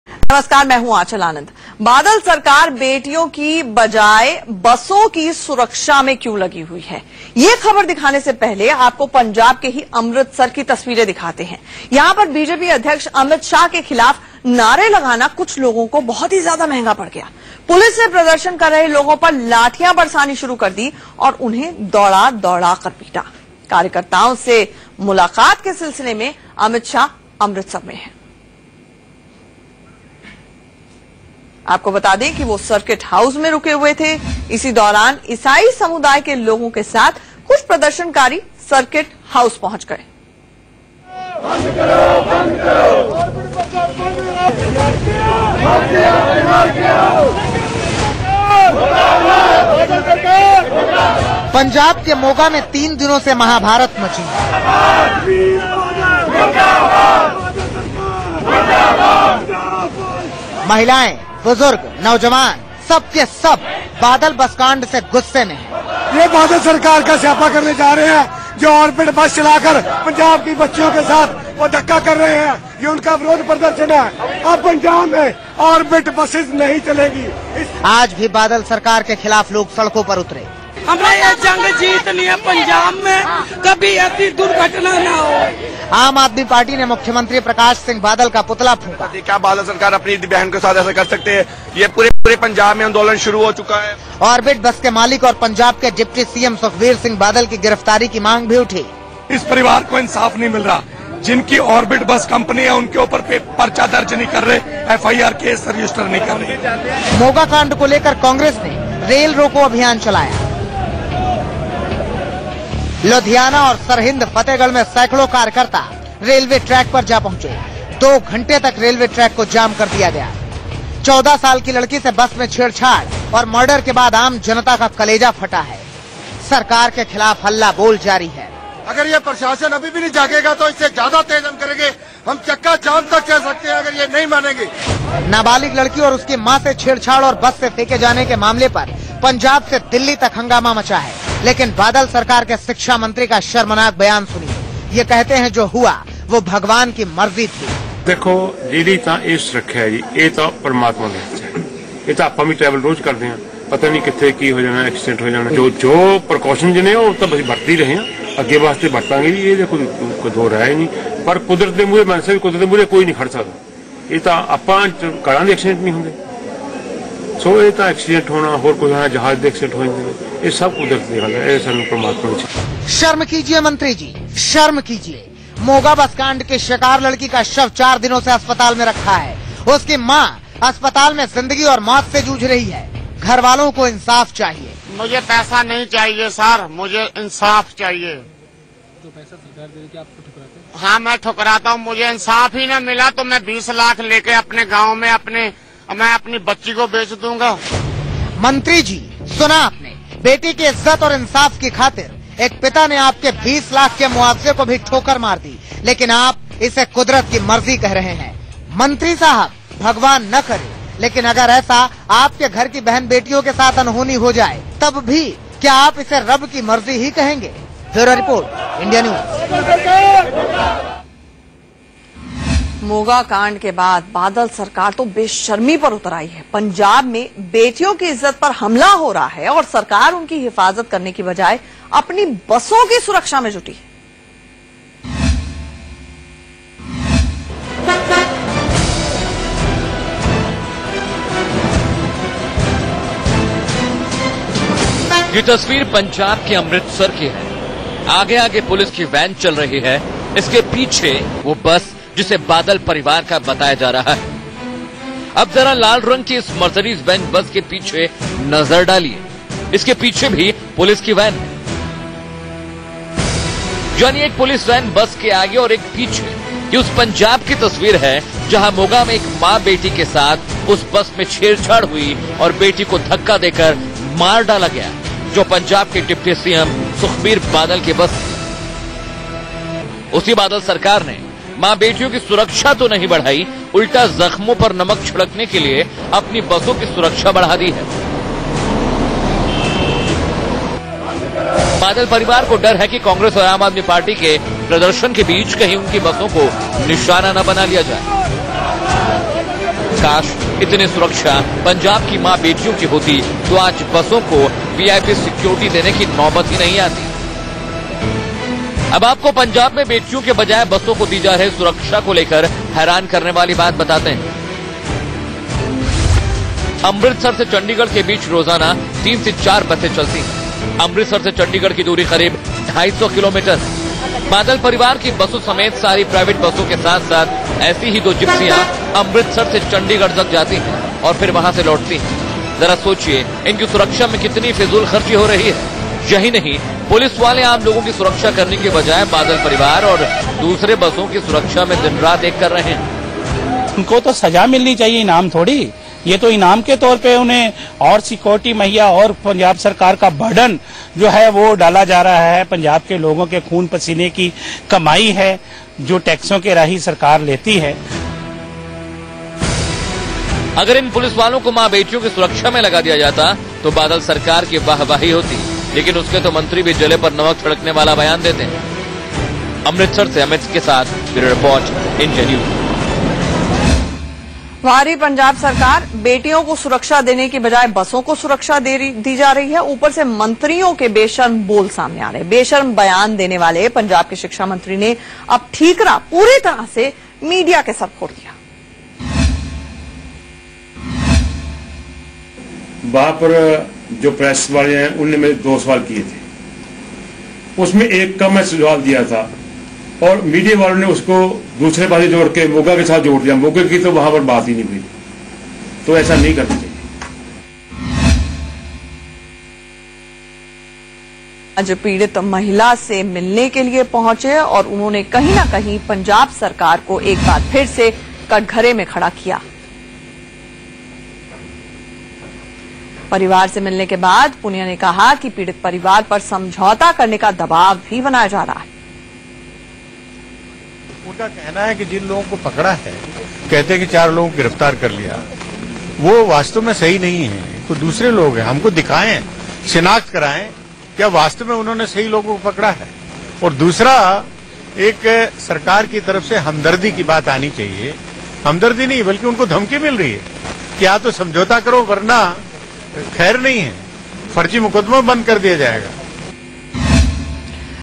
We demand FIR against Badal, they offer compromise, says Moga molestation victim's father
The family of 13-year-old Arshdeep Kaur who died after being pushed out of a bus here late Wednesday while resisting an alleged molestation bid has refused a postmortem and cremation. They are demanding registration of a case against Deputy Chief Minister Sukhbir Badal, who owns Orbit Company, to which the bus belonged. Arshdeep’s father Sukhdev Singh also demanded that the four accused arrested by the police be paraded in front of his wife, who witnessed the alleged molestation. “Police have till now not even shown them to us for identification,” Sukhdev Singh said. Arshdeep’s mother, who was also pushed out of the bus, is hospitalized.